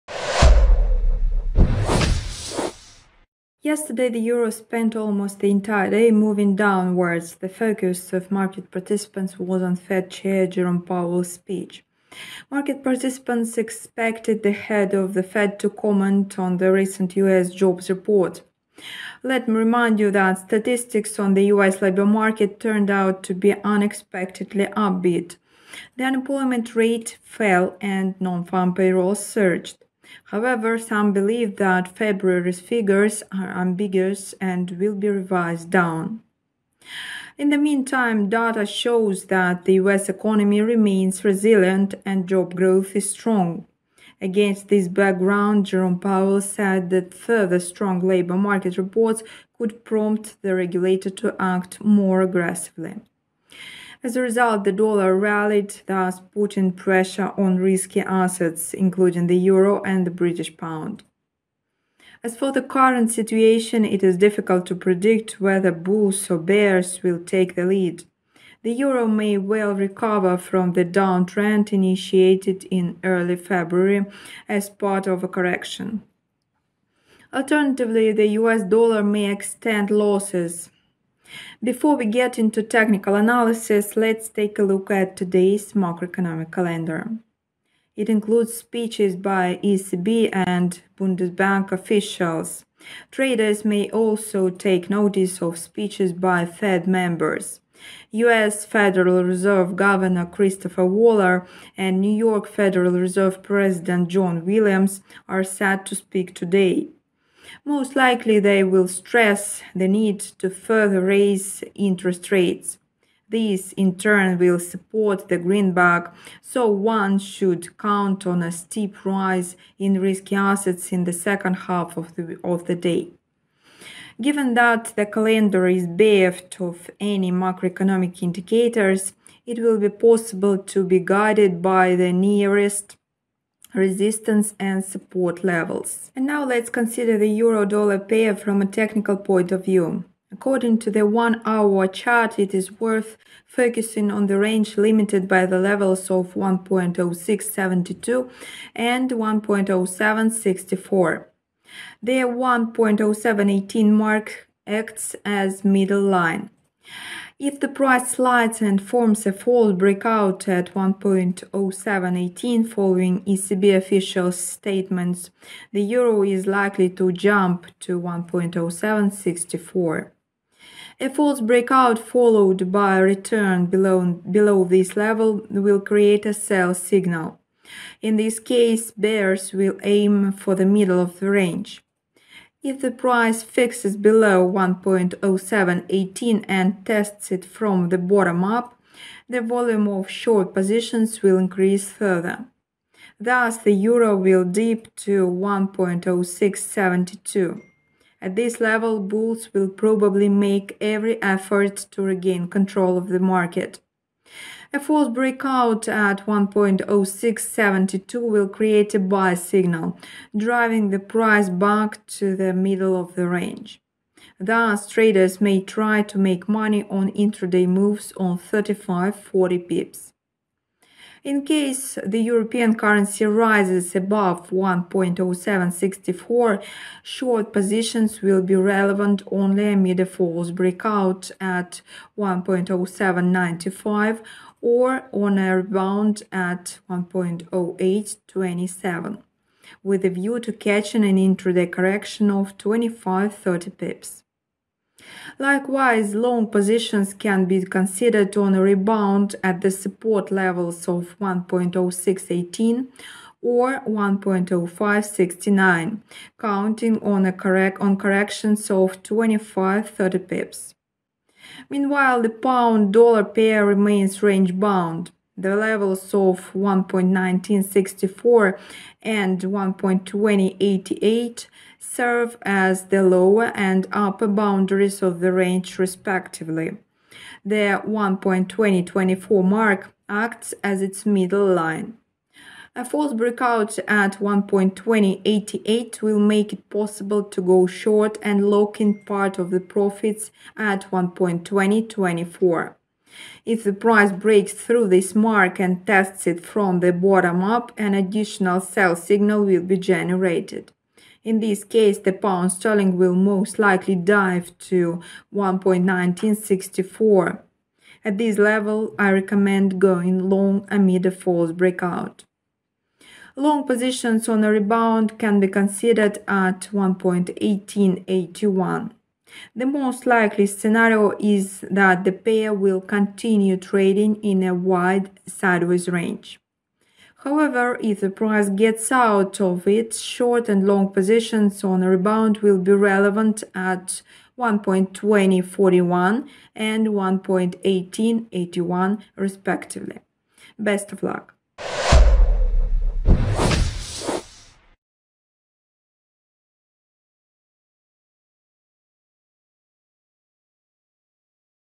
EUR/USD, GBP/USD Forecast for February 8, 2023. Euro, sterling dip after Powell speech.
00:00 Intro
01:21 Today’s news and events
02:16 EUR | USD
04:21 GBP | USD